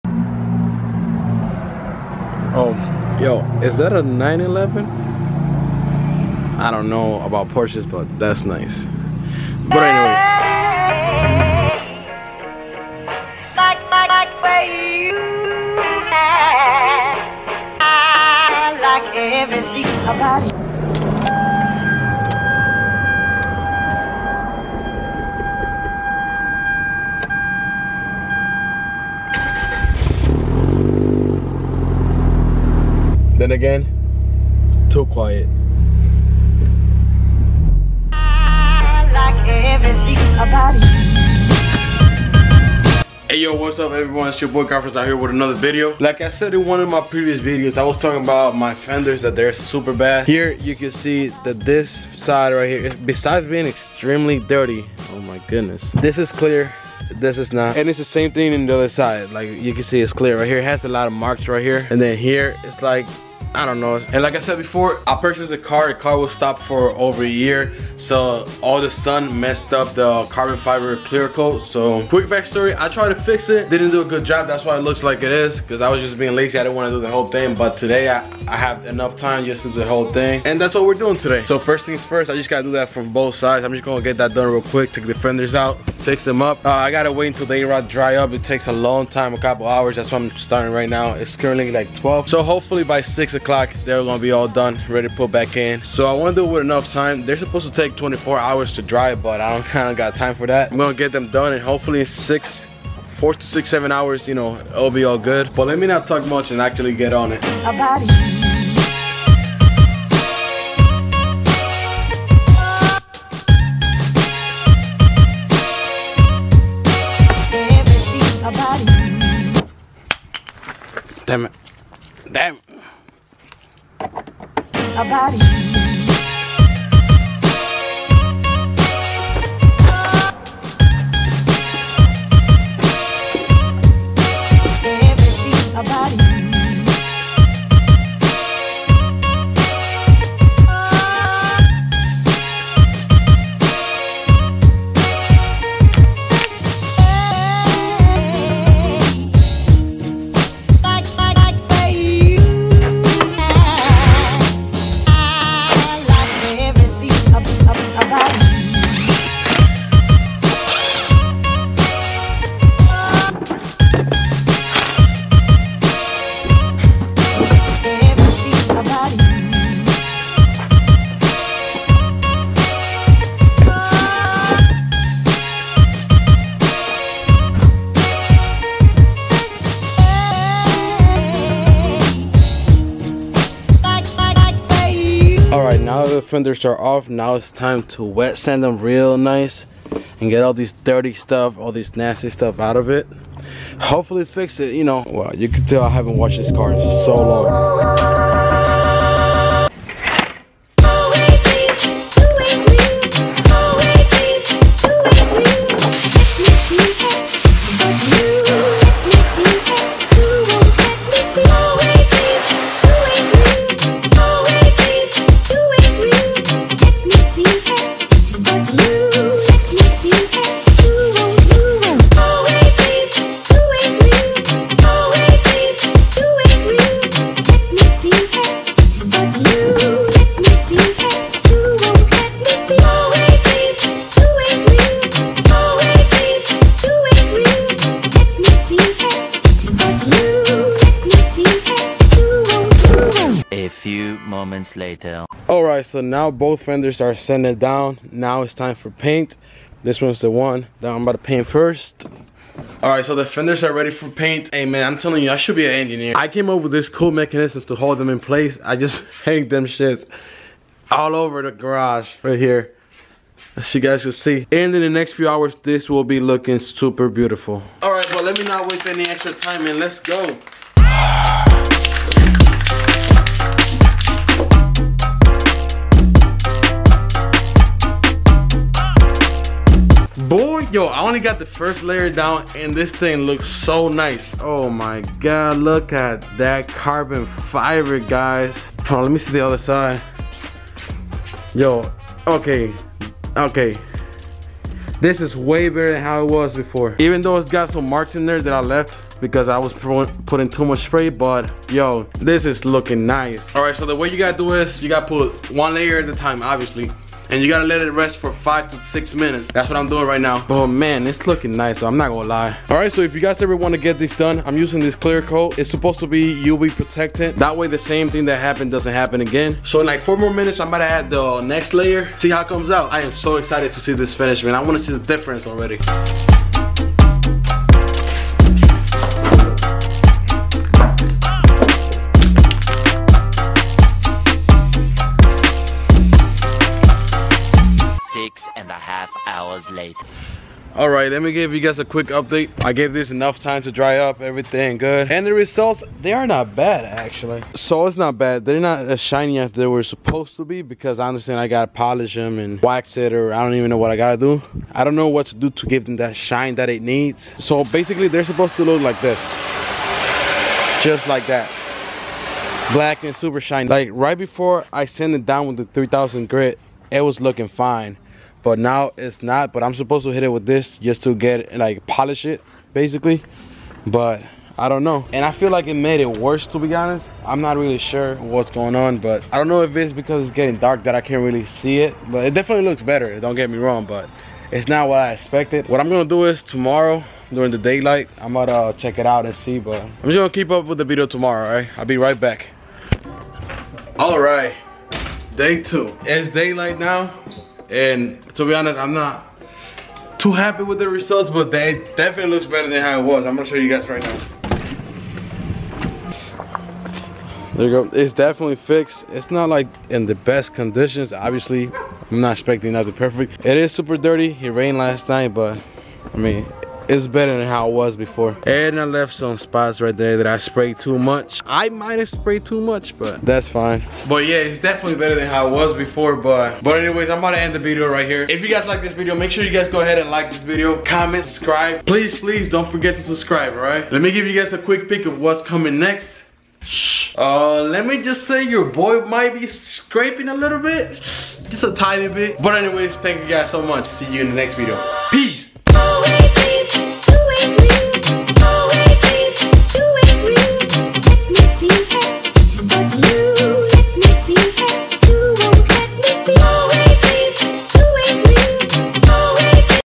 Carbon fiber fenders restored *NOT HOW I EXPECTED* !!
I tried restoring the carbon fenders but it didn't come out as I thought it would :(

carfrans_25